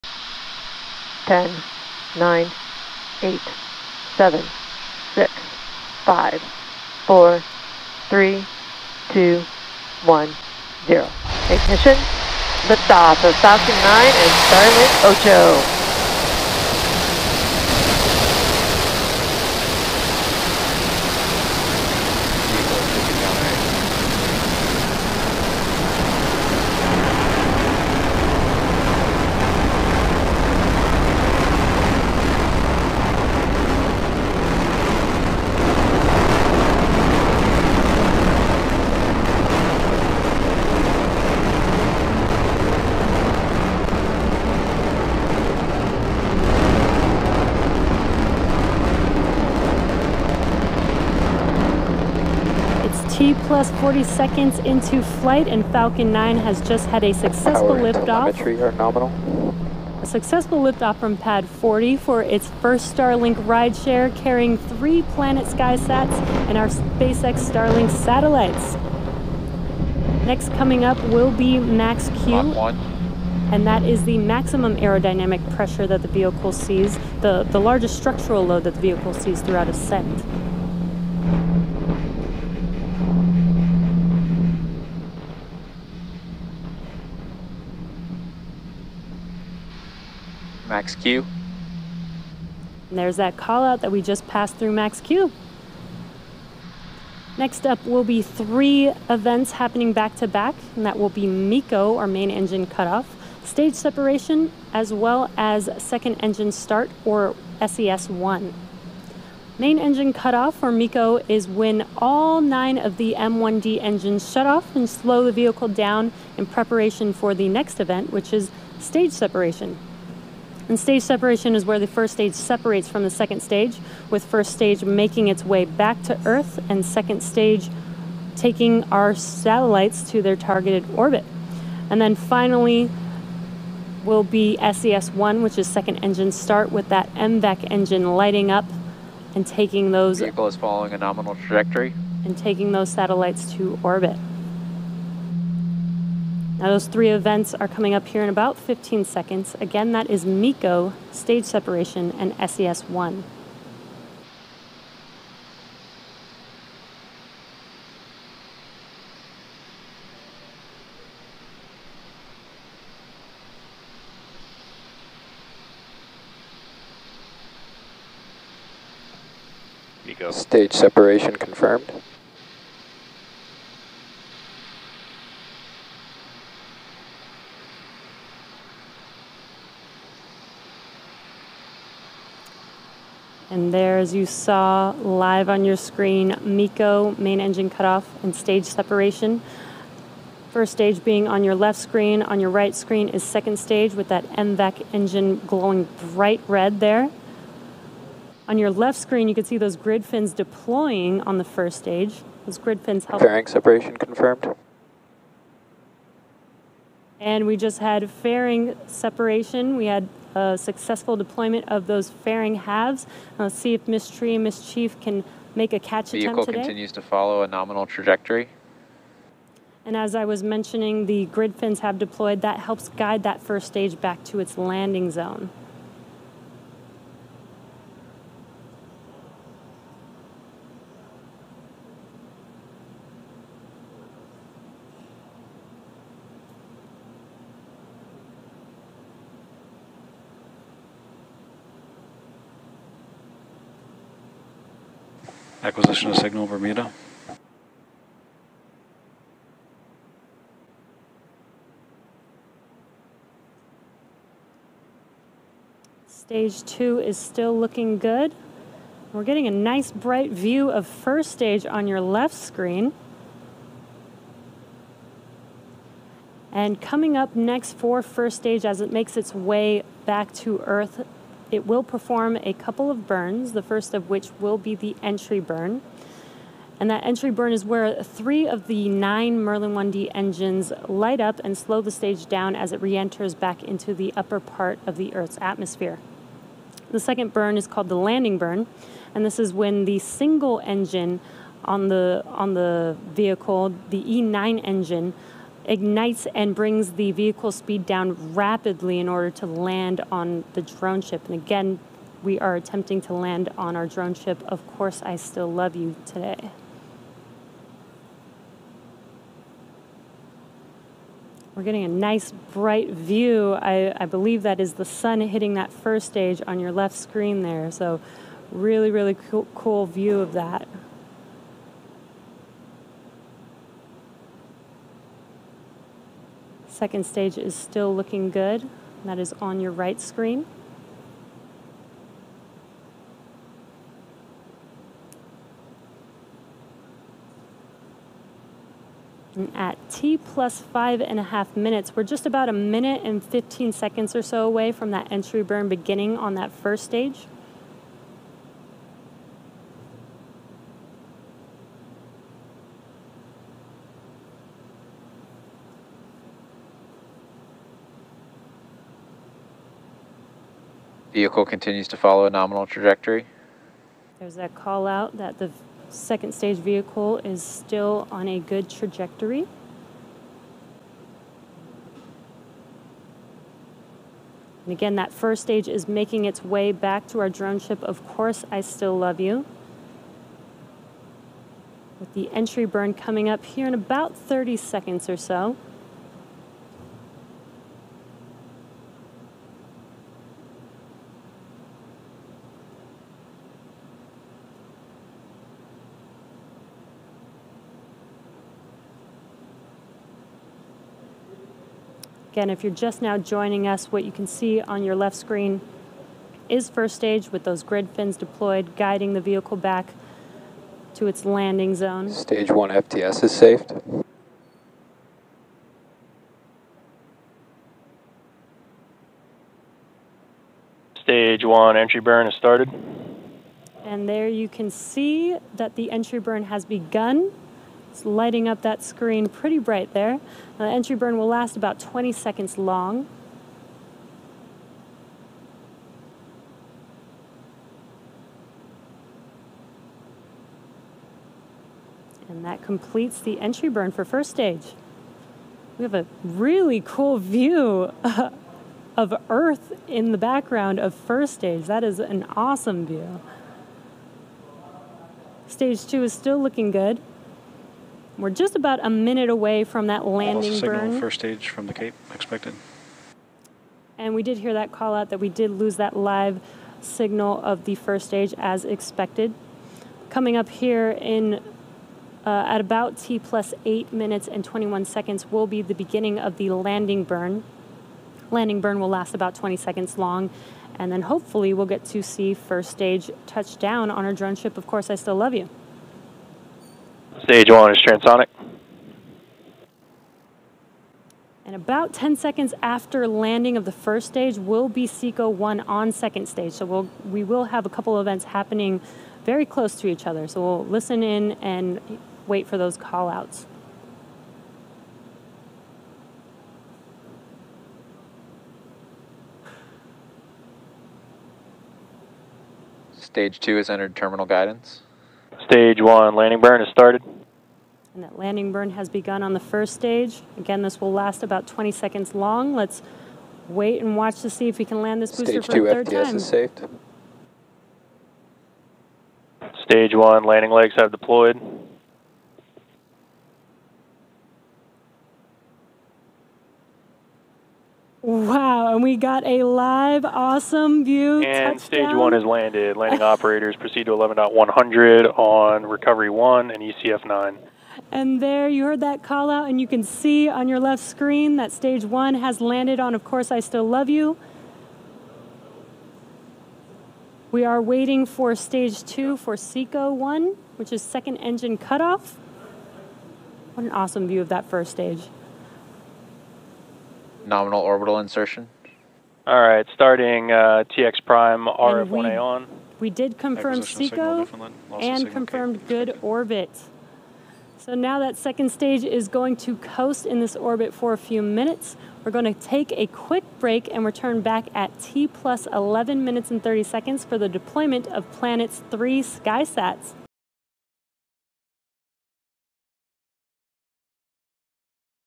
SpaceX Launches 58 Starlink Satellites Into Low Earth Orbit
SpaceX launched 58 more Starlink internet satellites. It is the company's 9th orbital launch of the year so far and marks their first SmallSat Rideshare Program launch.